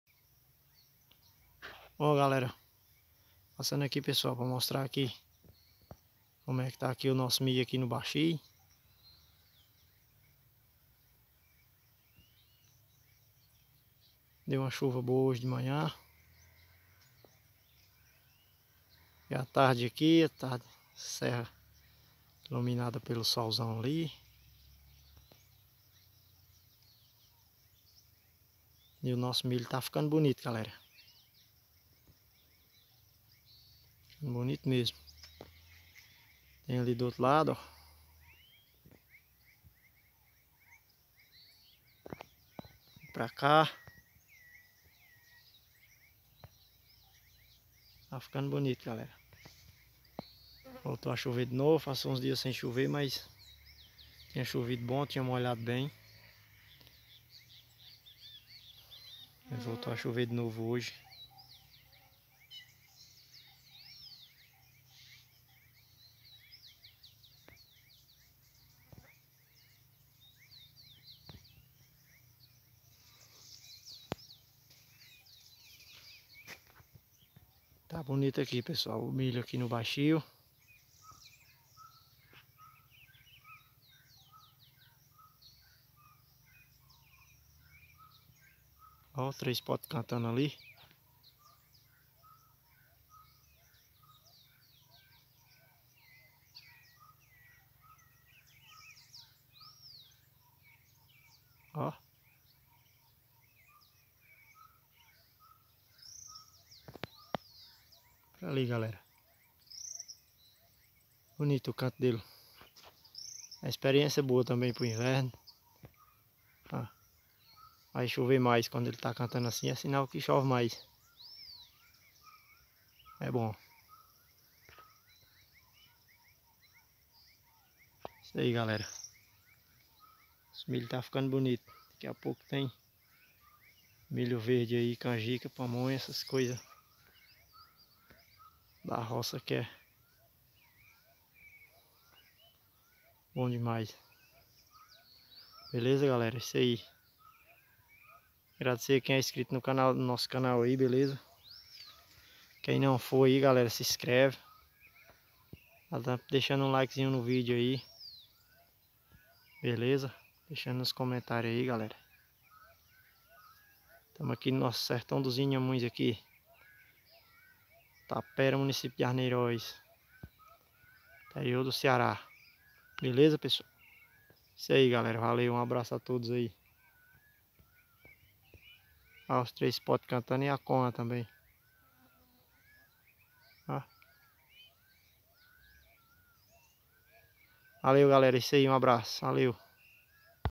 Plantação de milho no sertão nordestino no interior do estado do Ceará
O inverno no sertão ta bom demais, com muita chuva, e o agricultor já ta com as roças todas plantadas e milho está muito lindo, roça de milho no sertão cearense, interior da cidade de Arneiroz